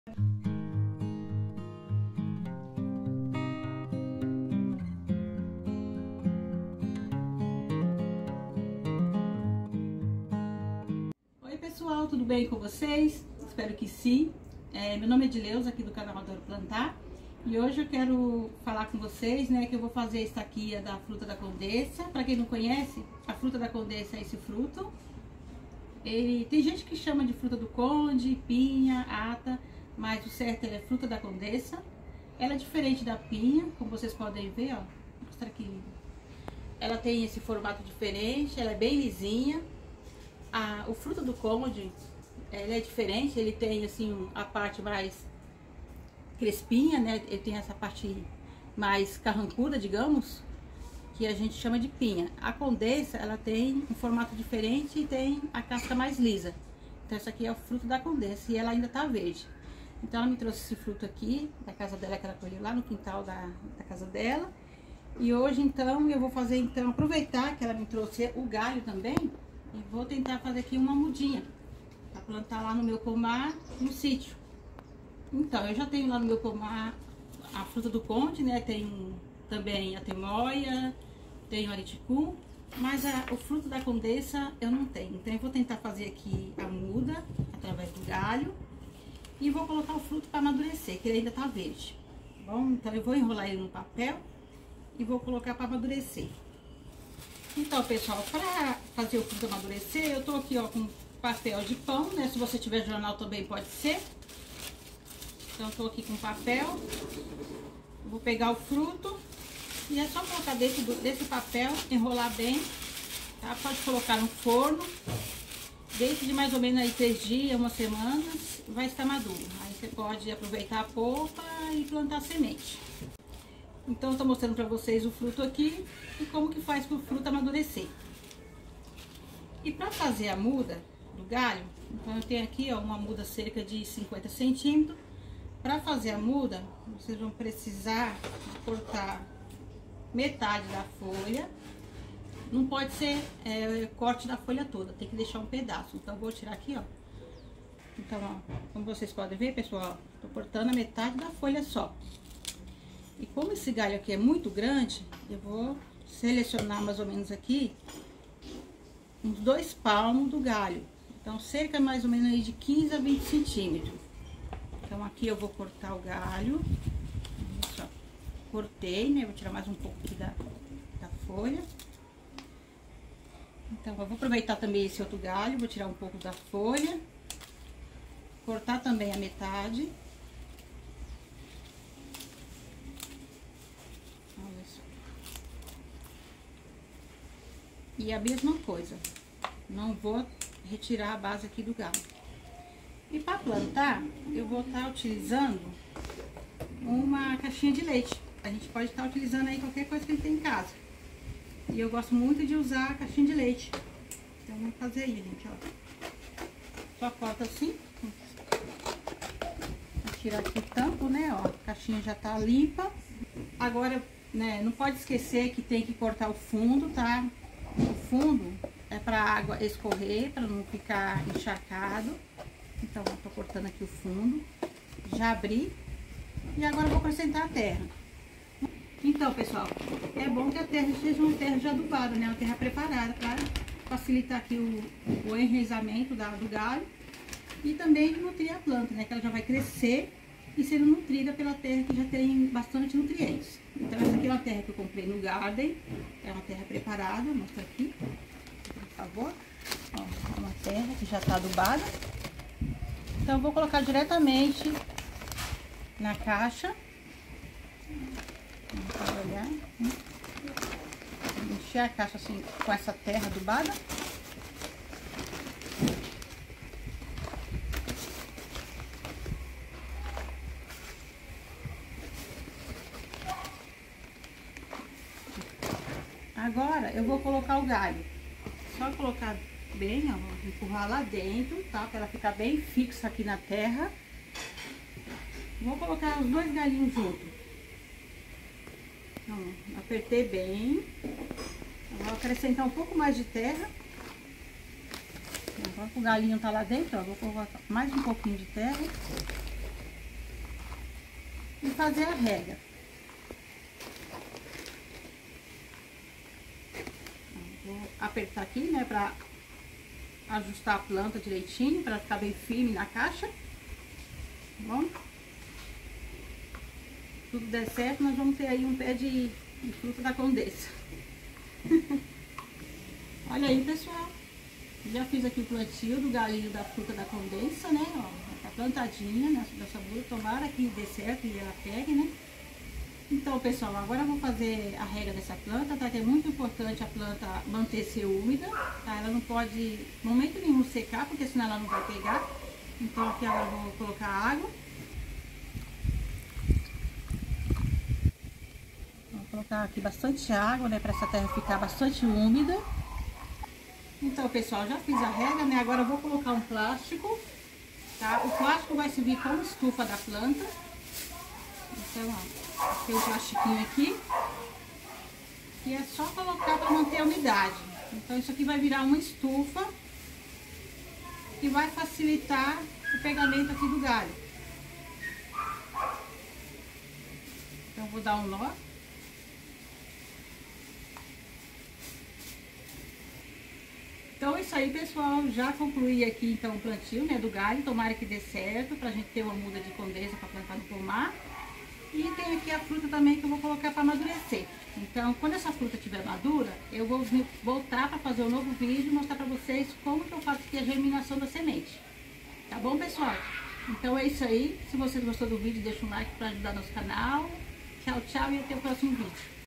👉COMO FAZER MUDAS DA FRUTA DA CONDESSA, TECNICA SIMPLES!!! 👍🍒🌿💚 COMO PLANTAR GALHO DE FRUTIFERA!
Neste video estou mostrando como fazer mudas de fruta da condessa serve para qualquer frutifera,